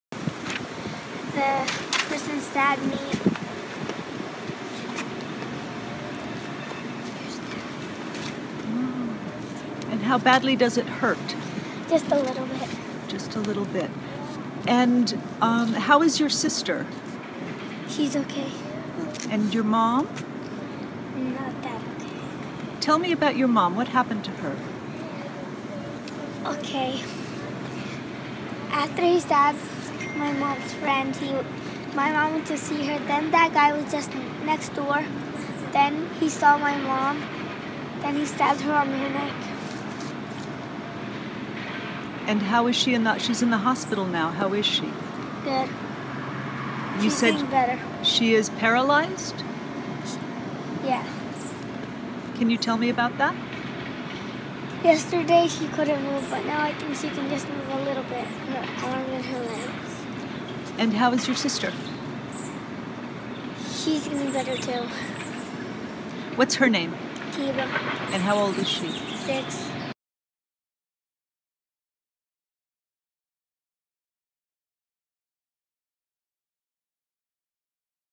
Eight-year-old boy describes being stabbed Saturday night in Boise
Zine Jalil, 8, was stabbed Saturday night along with his mother and sister. Jalil shares the status of his condition and his mother's condition with Idaho Statesman's Maria L. La Ganga.